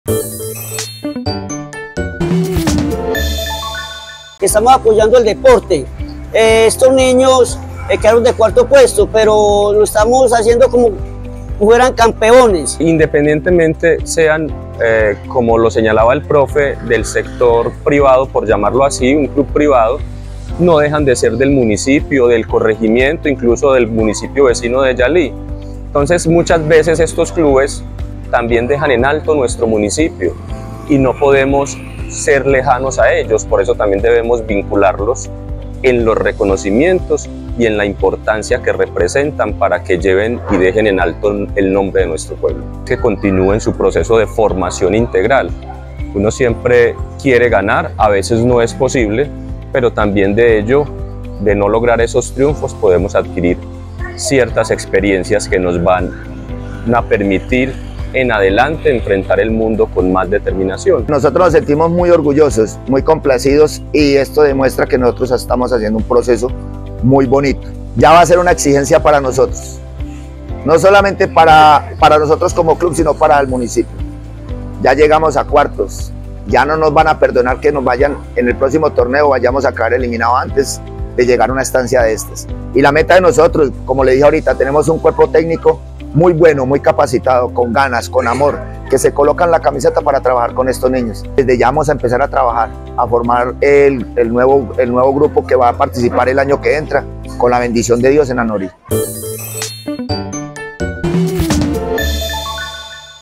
Reconocimiento CORDEPOR LA DULZURA
La Administración Municipal y el concejal Jorge Henao realizaron un emotivo reconocimiento a los deportistas del 𝗖𝗹𝘂𝗯 𝗗𝗲𝗽𝗼𝗿𝘁𝗶𝘃𝗼 𝗟𝗮 𝗗𝘂𝗹𝘇𝘂𝗿𝗮 por su valerosa participación en el zonal del Baby Fútbol, ocupando el 4° lugar y destacándose entre otras cosas, por el juego limpio. ¡𝗠𝗲𝗿𝗲𝗰𝗶𝗱𝗮𝘀 𝗙𝗲𝗹𝗶𝗰𝗶𝘁𝗮𝗰𝗶𝗼𝗻𝗲𝘀!